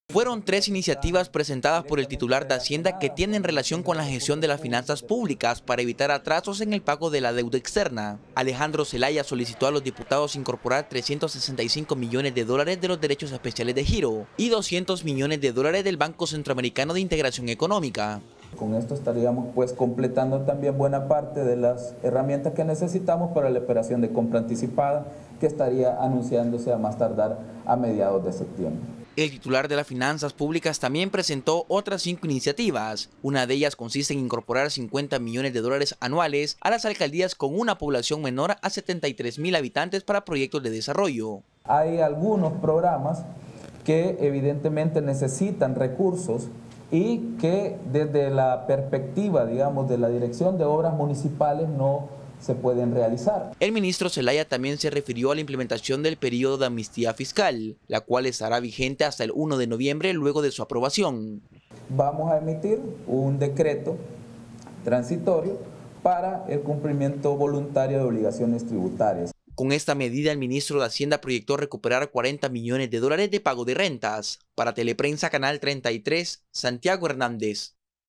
Teleprensa33 | Fueron presentadas 3 iniciativas de gestión de pasivos
El ministro de Hacienda presentó este martes un pliego de iniciativas para la compra anticipada de la deuda y entrega de 50 millones de dólares para las alcaldías.